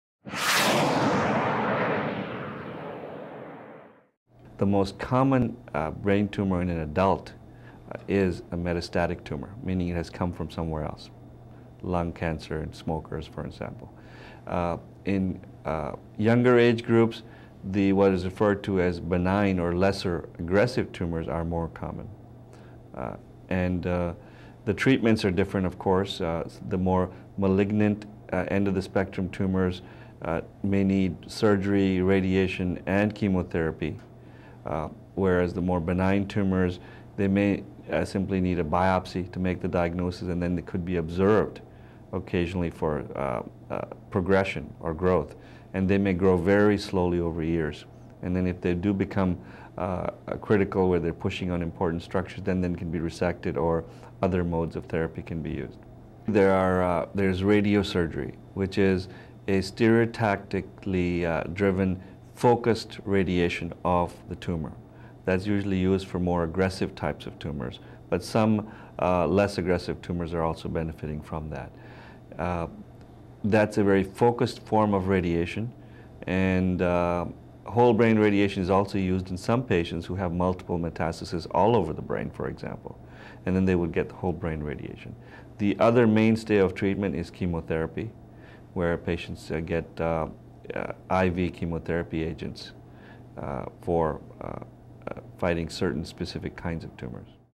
How do you treat a brain tumor?
How do you treat a brain tumor? The most common brain tumor in adults is a metastatic tumor, meaning it came from somewhere else, such as lung cancer.  In younger people, benign or lesser tumors are more common.  The type of treatment varies based on the severity of the tumor.  For tumors on the malignant side of the spectrum, surgery, radiation, chemotherapy, or even all three may be needed to treat it.  For more benign tumors, they may only require a biopsy in order to diagnose the tumor.  Then it might only  be required to observe the tumor and monitor the growth, as it may only grow at a very slow rate.  If it begins to push on vital structures, then another form of treatment can be implemented such as radiotherapy.